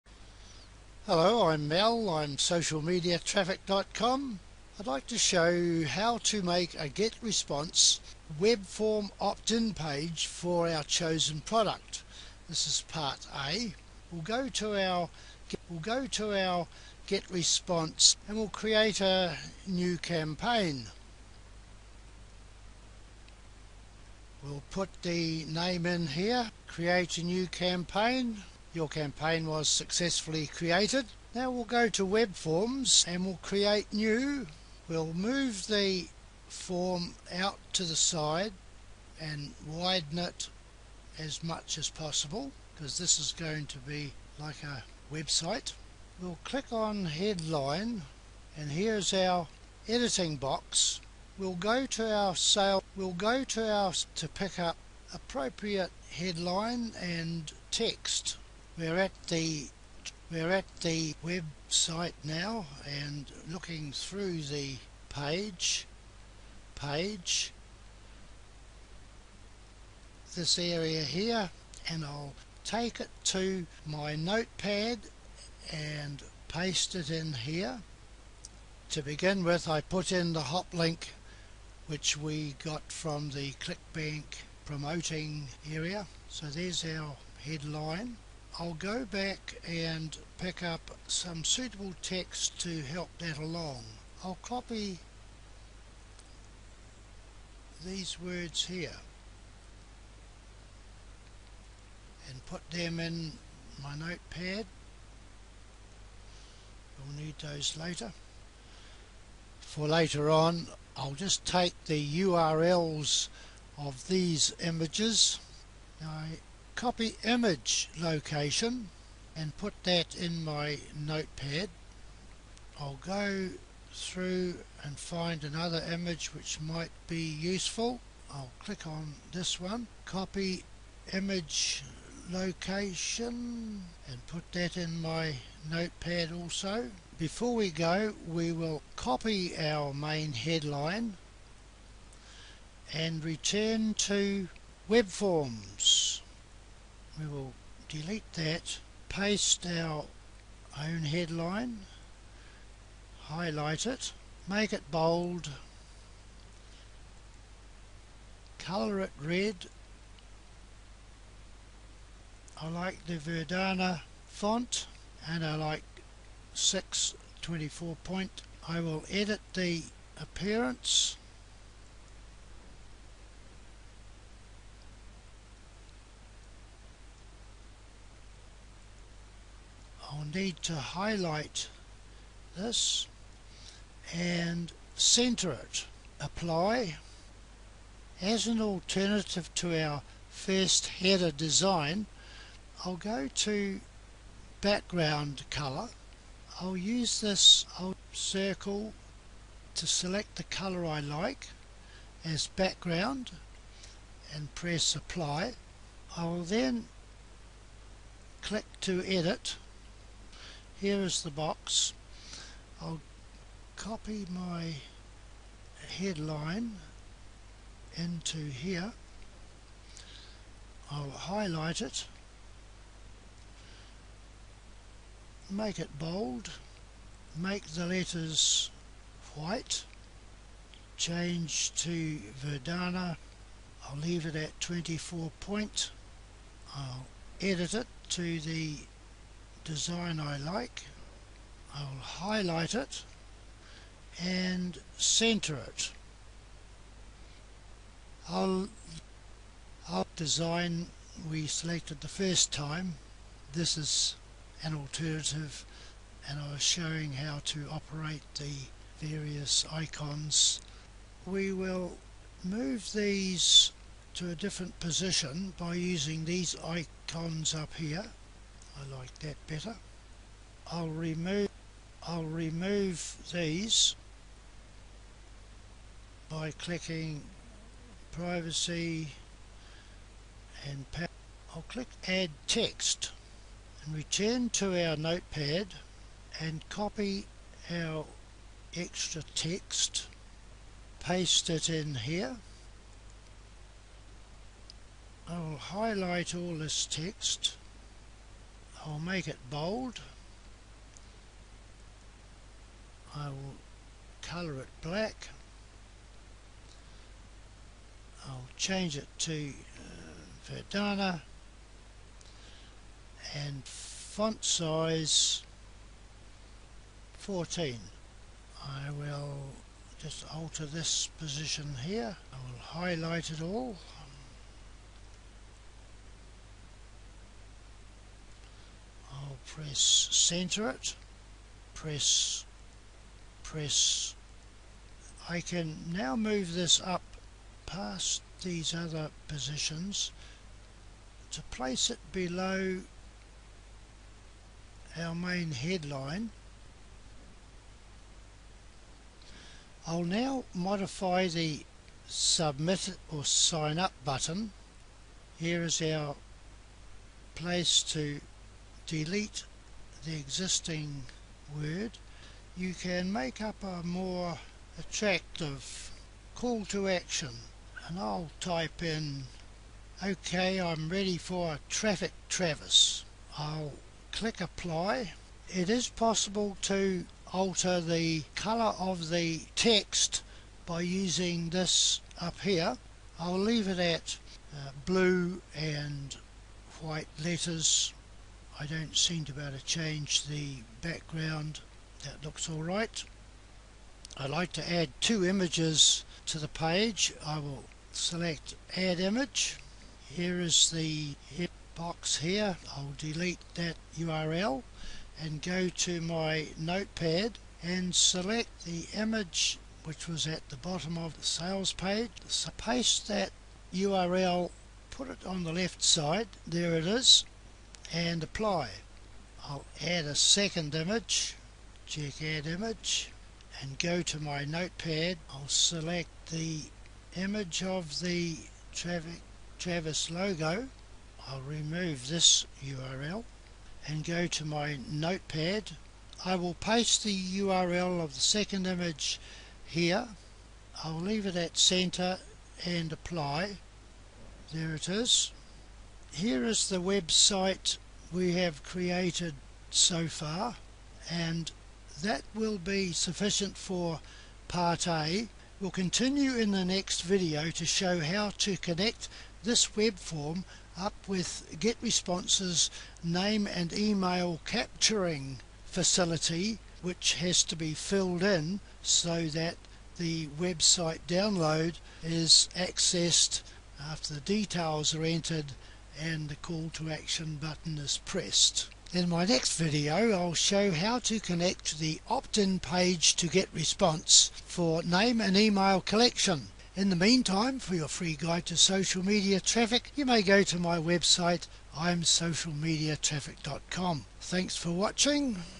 How to make a GetResponse Webform Optin page for our Product -- Part A
Login to Getresponse autoresponder -- Create campaign name -- Traffic Travis
Create webform -- choose the plain template & spread it wide
Open a note pad & paste in the Clickbank Hoplink we saw how to  get in the previous video
For the Header & for text to go below  go to the Traffic Travis sales page to copy a headline & text to help it
Paste it into the Note pad -- Right click on Images  to get their URL -- my system  gives it via Copy Image location
Paste both URLs into Note pad -- copy Headline & go back to Webform
Click on form type,  then Header  - background colour red -- click for edit box -- from notepad copy headline & paste into box -- Verdana -- 6 pt -- bold -- center it - apply
How to connect a  GetResponse Webform Optin page with Autoresponder – Part B